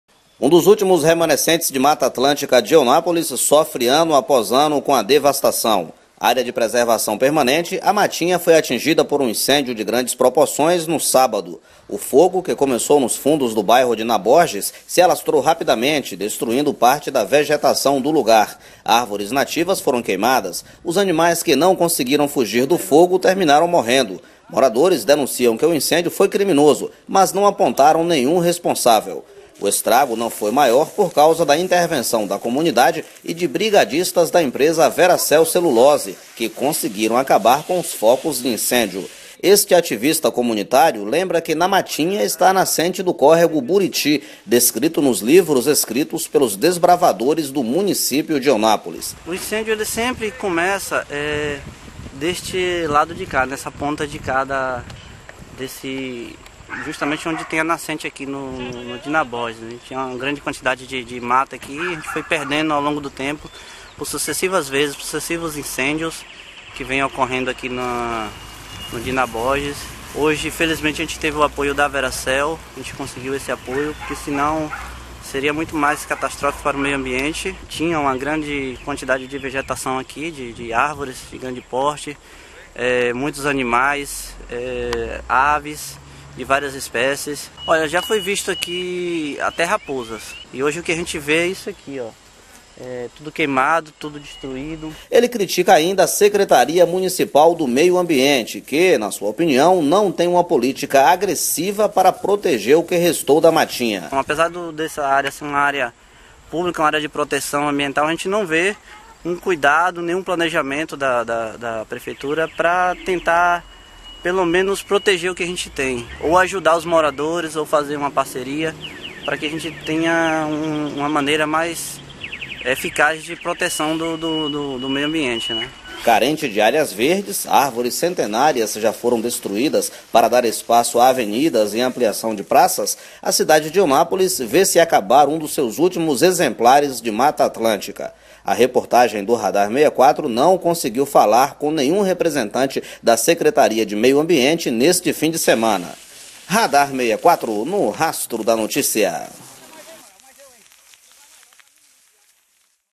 Fogo que destruiu parte da Matinha pode ter sido criminoso
Um dos últimos remanescente de mata atlântica de Eunápolis sofre ano após ano com a devastação.
Área de preservação permanente, a Matinha foi atingida por um incêndio de grandes proporções no sábado (15).
O fogo, que começou nos fundos do bairro Dinah Borges, se alastrou rapidamente, destruindo parte da vegetação do lugar.
Árvores nativas foram queimadas. Os animais que não conseguiram fugir do fogo terminaram morrendo.
Moradores denunciam que o incêndio foi criminoso, mas não apontaram nenhum responsável.
O estrago não foi maior por causa da intervenção da comunidade e de brigadistas da empresa Veracel Celulose, que conseguiram acabar com os focos de incêndio.

Saiba mais em nossa página na internet: RADAR64 - no rastro da notícia.